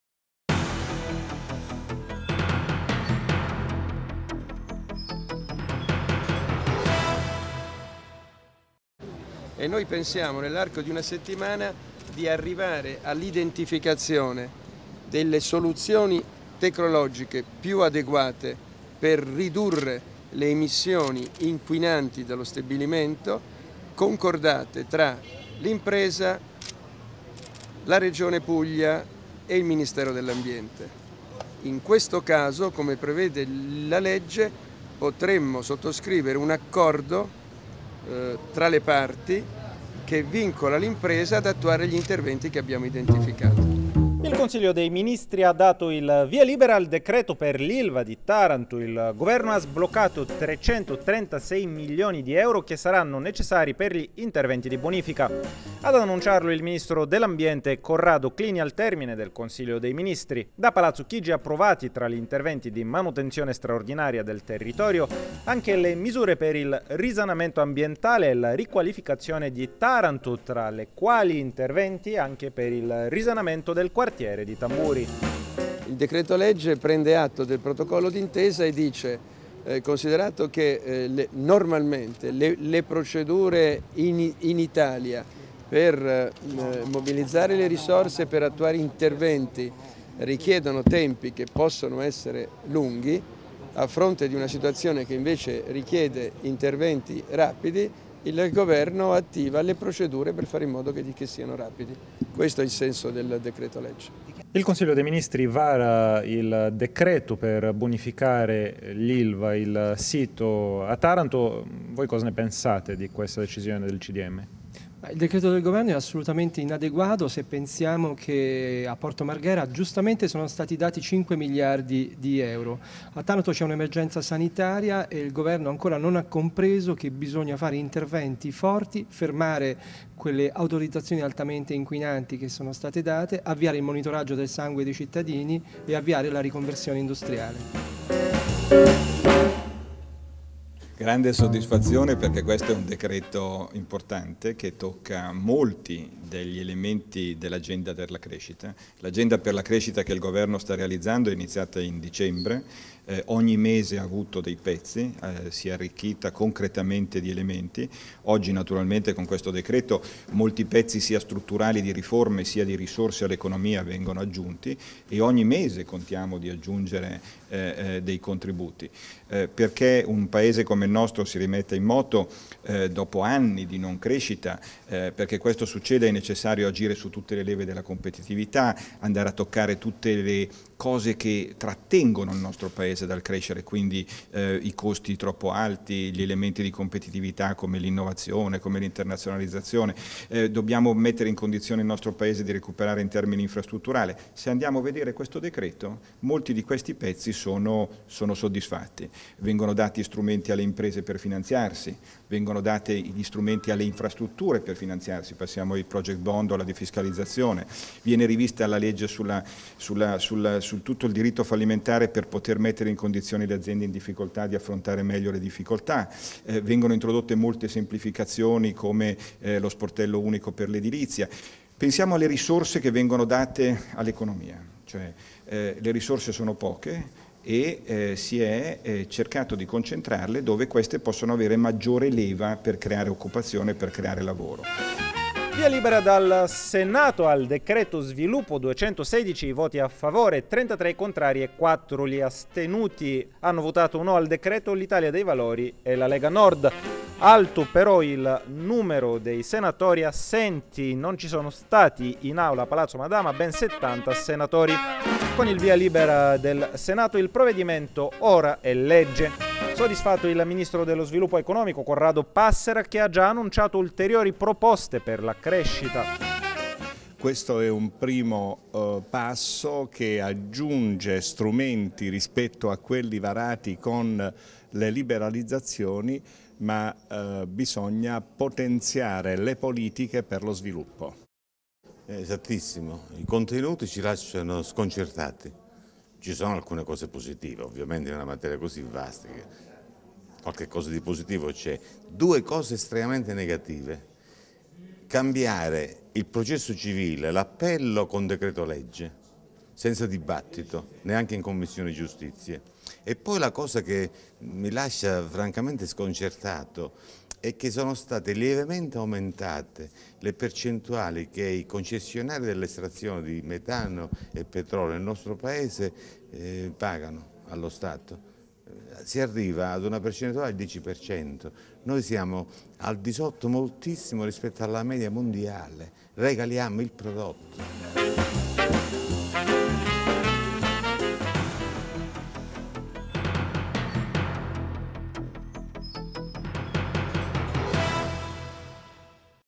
3-08-12 LA GIORNATA POLITICA
Governo vara il decreto per bonificare il sito del'Ilva - con intervento del ministro Clini, il commento di Angelo Bonelli
- Il Senato approva il decrero sviluppo - l'intervista al ministro Passera, le reazioni di Bubbico (Pd) e Li Gotti (Idv)